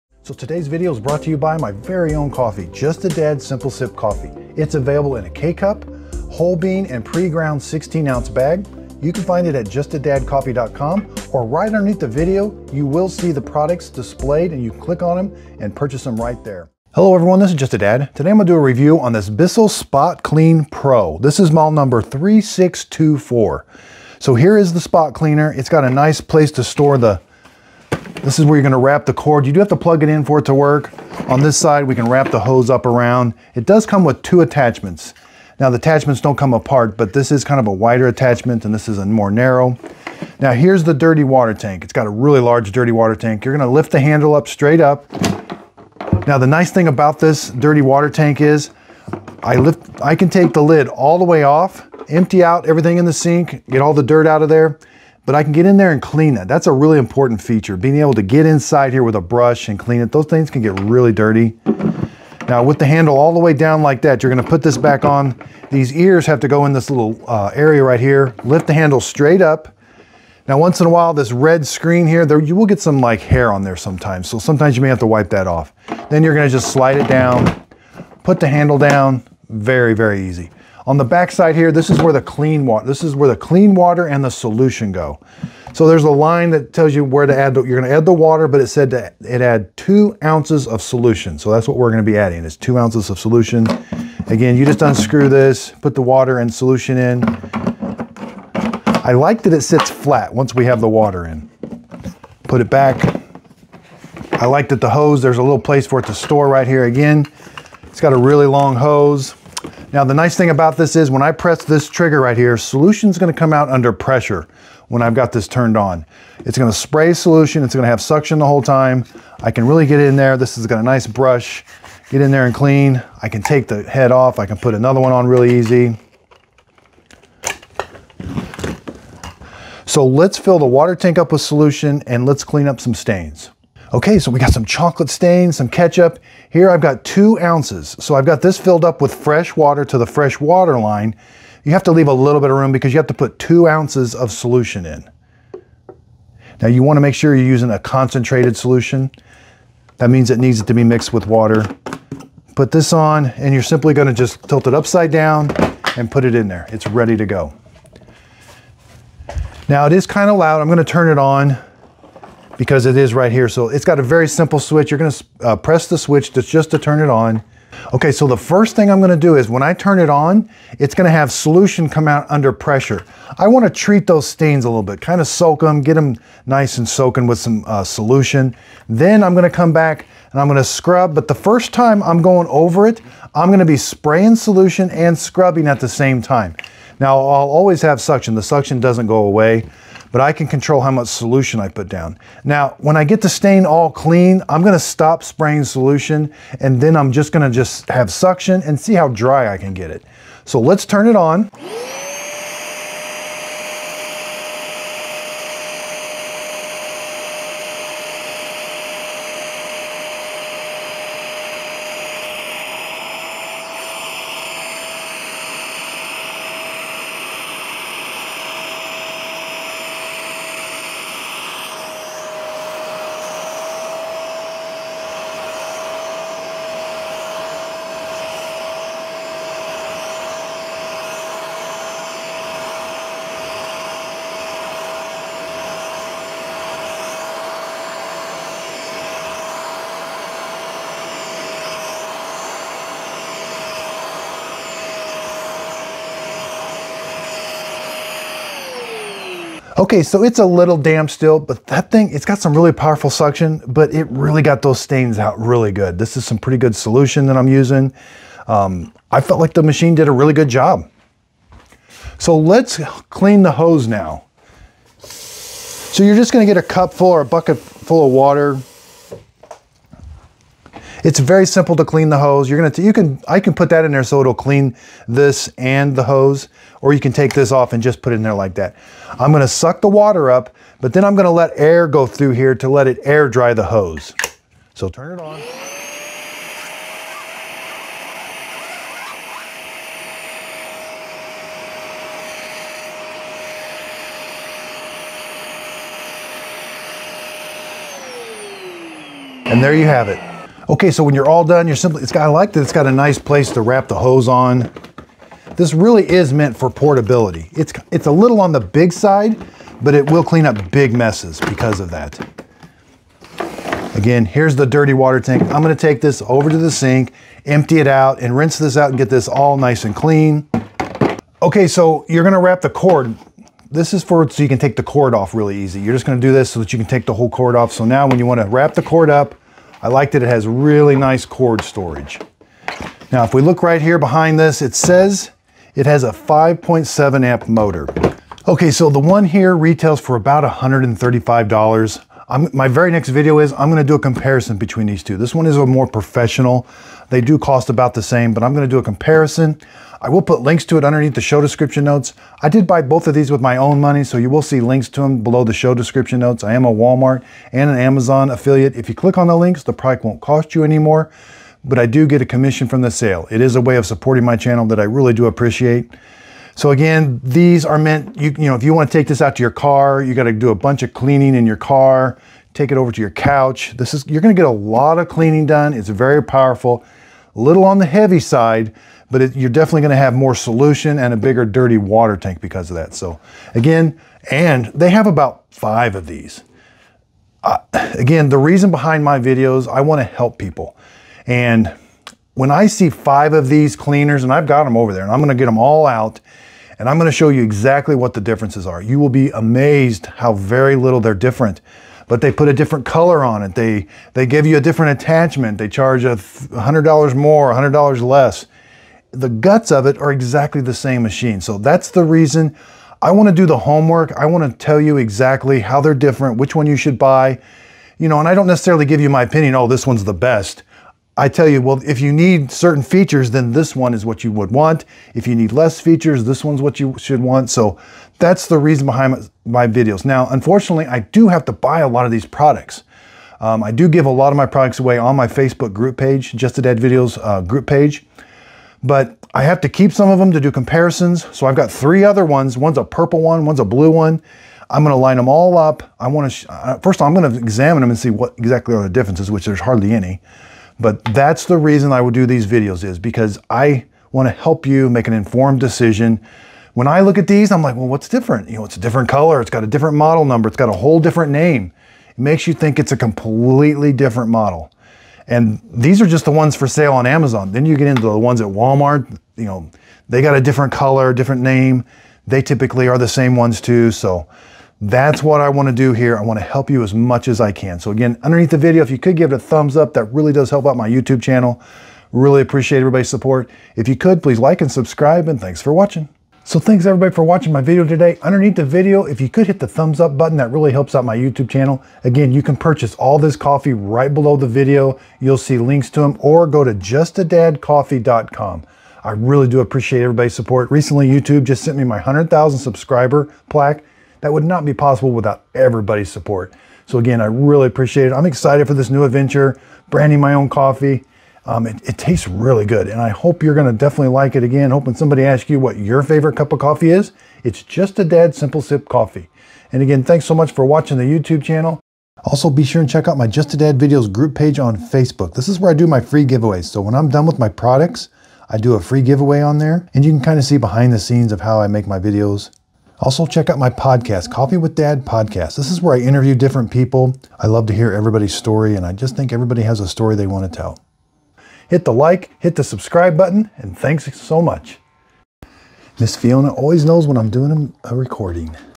Works Great! Bissell 3624 Spot Clean Portable Carpet Cleaner REVIEW
Bissell 3624 Spot Clean Professional Portable Carpet Cleaner - Corded (paid link) walmartpartner Features of cleaner and how to use. Demonstration of cleaning up a mess on carpet. How to clean the hose.